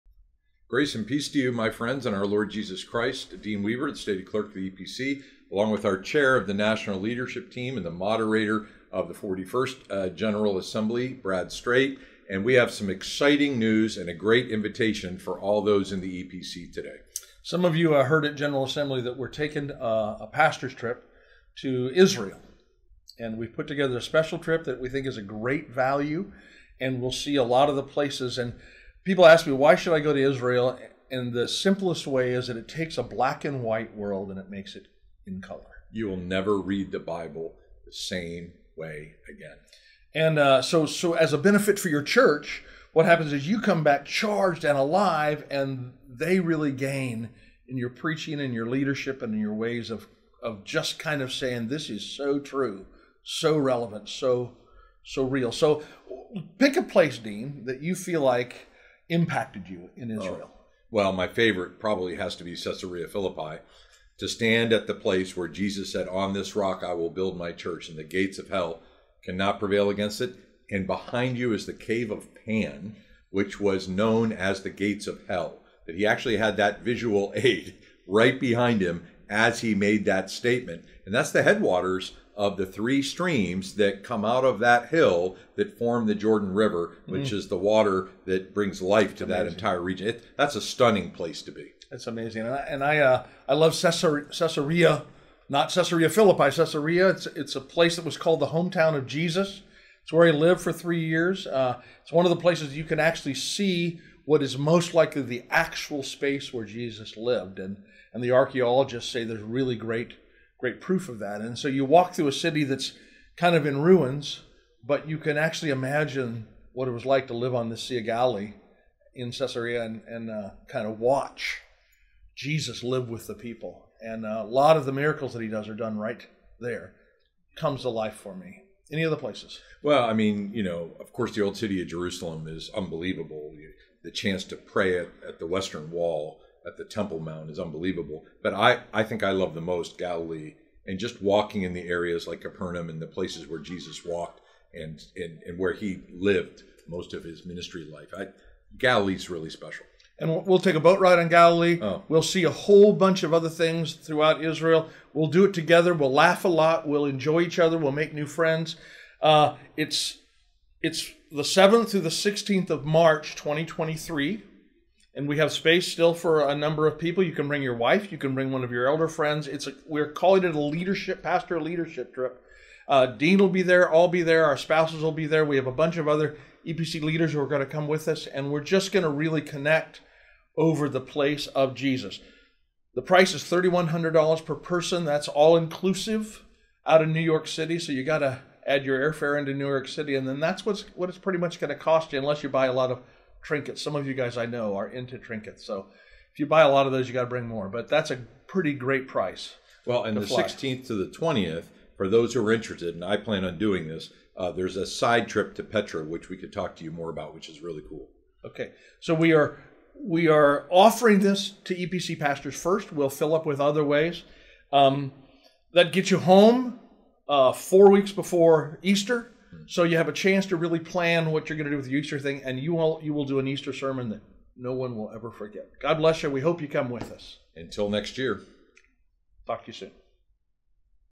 Israel Tour 2023 Promo
Join EPC Stated Clerk Dean Weaver and National Leadership Team Chairman Brad Strait for a tour of the Holy Land, March 7-16, 2023.